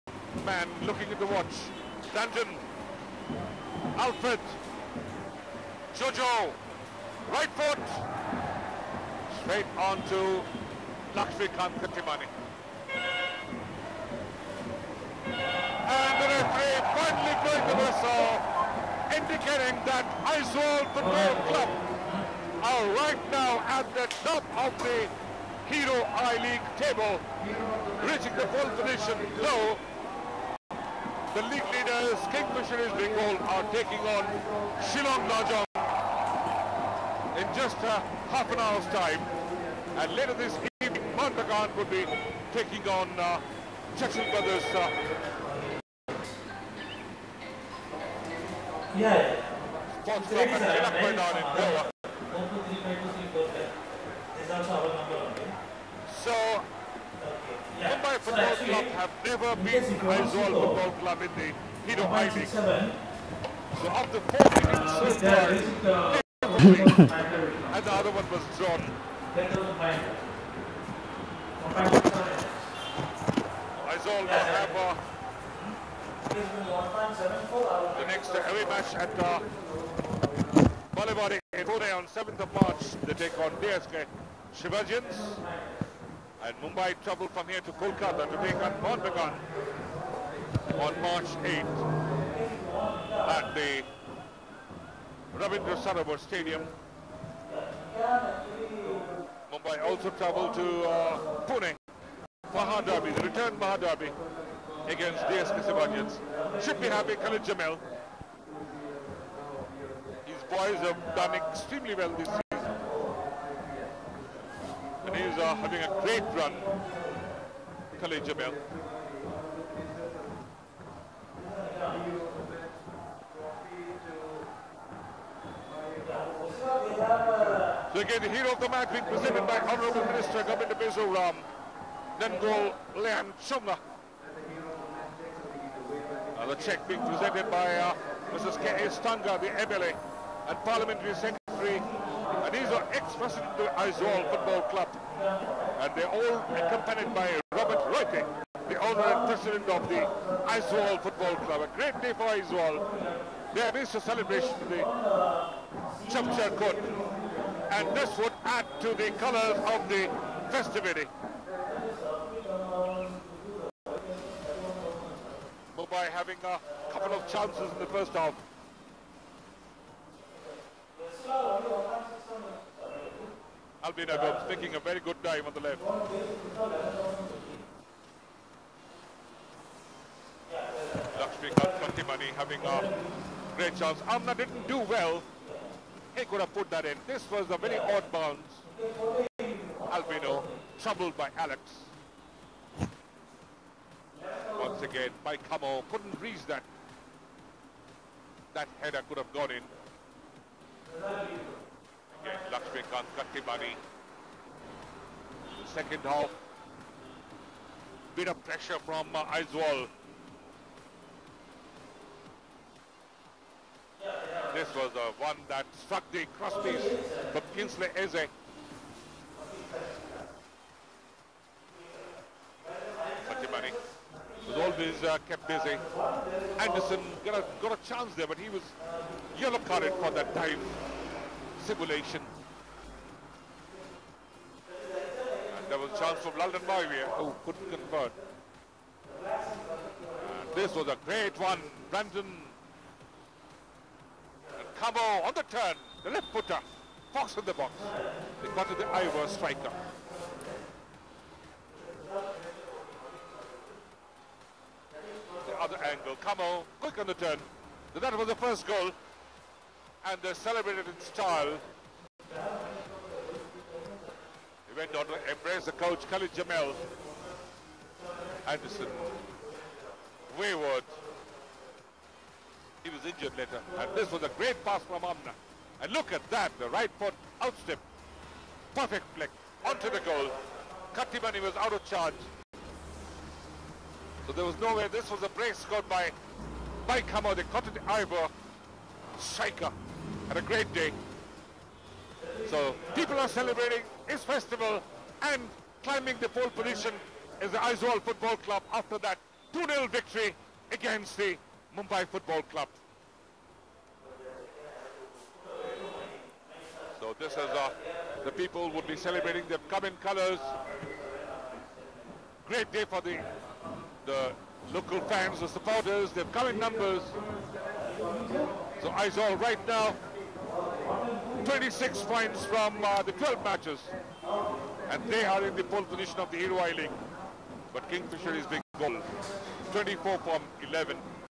Aizawl FC 2-0 Mumbai FC (Hero I-League 2017) Highlights @Rajiv Gandhi Stadium, Mualpui
3/3/2017 (Saturday) Aizawl FC Played against Mumbai FC and won by 2-0 (FT).
2 goals scored from Aizawl FC striker Kamo and he has selected Man of the Match.

After celebration of CHAPCHAR KUT (2/3/2017) in Mizoram, Aizawl FC fans (Mizo's people) gathered again for "The People's Club" at Rajiv Gandhi Stadium, Mualpui. Aizawl.